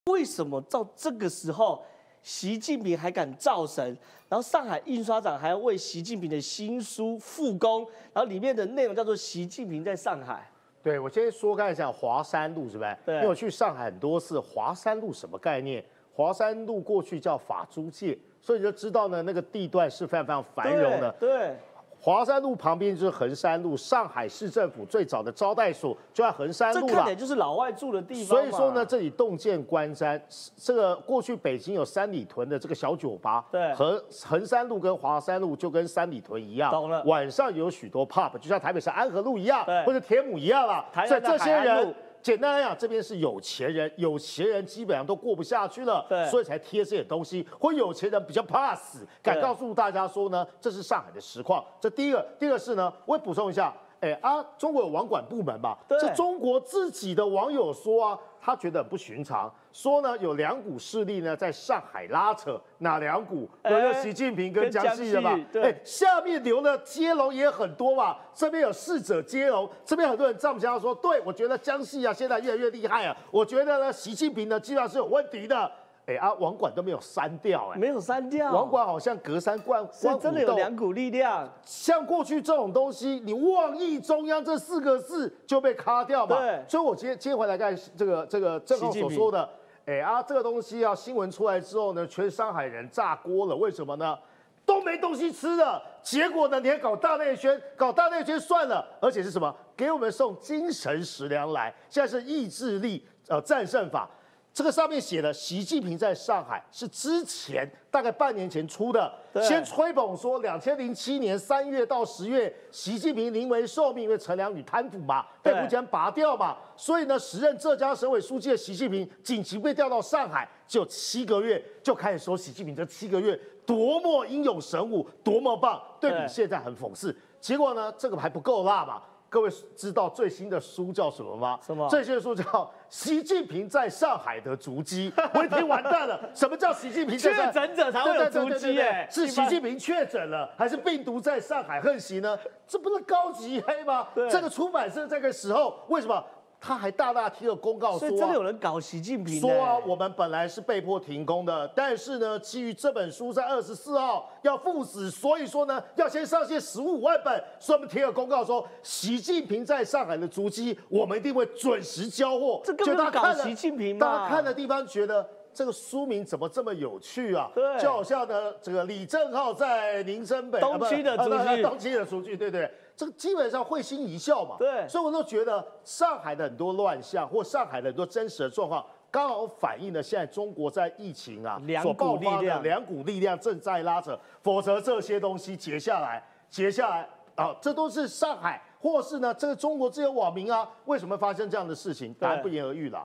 【94要客訴】中央黨校出版《習近平在上海》！書商大推《習近平上海足跡》！張宇韶：反動派暗示習確診？
主持人 李正皓｜台灣韜略策進協會副理事長 張宇韶

中官媒央視網大推系列文：《習近平戰「疫」兵法》
有點酸？中共中央黨校閃亮出版《習近平在上海》
貫徹習論述！《應急管理體系和能力建設干部讀本》

友邦連結區